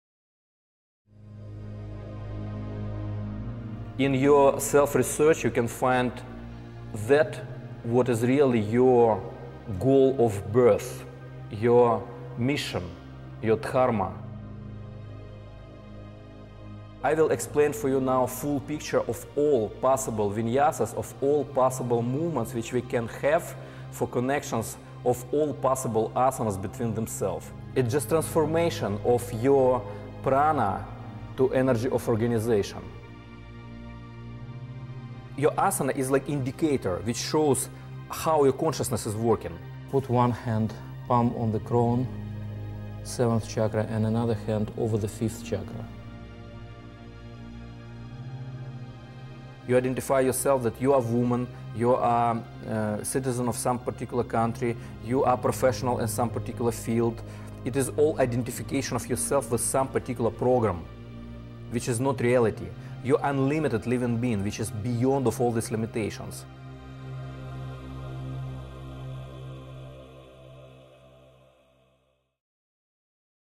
Trailer for Universal Theory of Yoga with Andrey Lappa
Universal Yoga is a systematic approach to spiritual evolution based on the key principles of classical yoga, creativity in practice and personal freedom. Andrey Lappa developed the Universal Yoga system after two decades of intensive study and practice with spiritual teachers throughout Asia. His theory lectures offer insightful and comprehensive knowledge and skills for harmonizing spiritual awakening through yoga with everyday life.